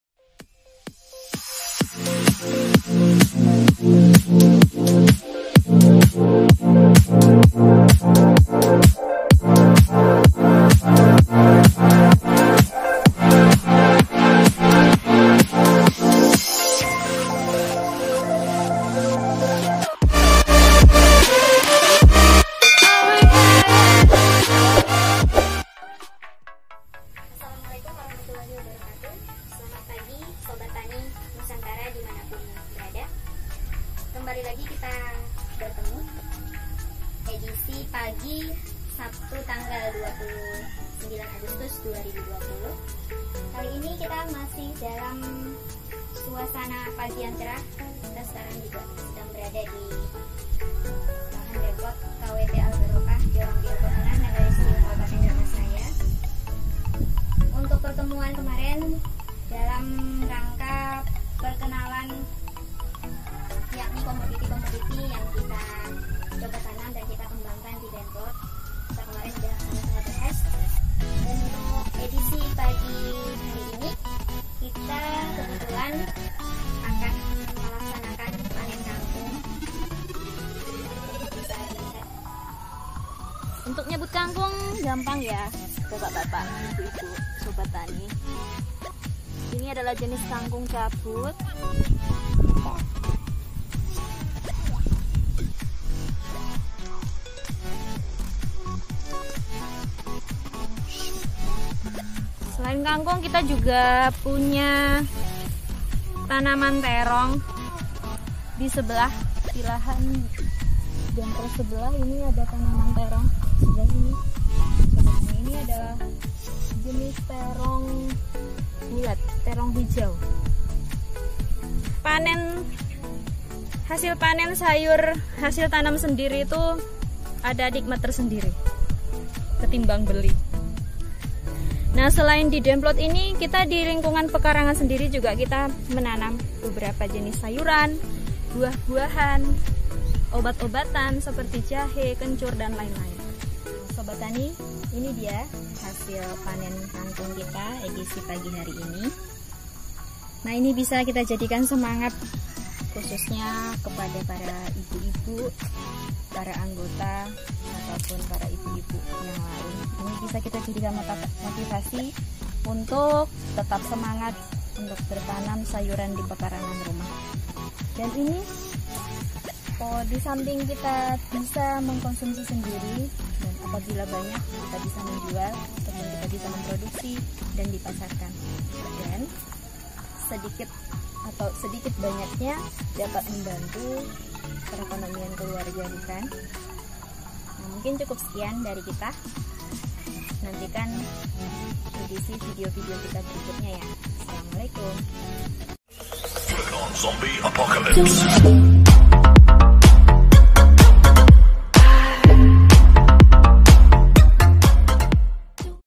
Sederhana Namun bermanfaat Edisi panen Sayuran KWT albarokah pangan sehat indonesia
Semoga menjadikan semangat petani muda indonesia.  Dalam menjaga pangan sehat.
Informasi lainya. LINK di bawah.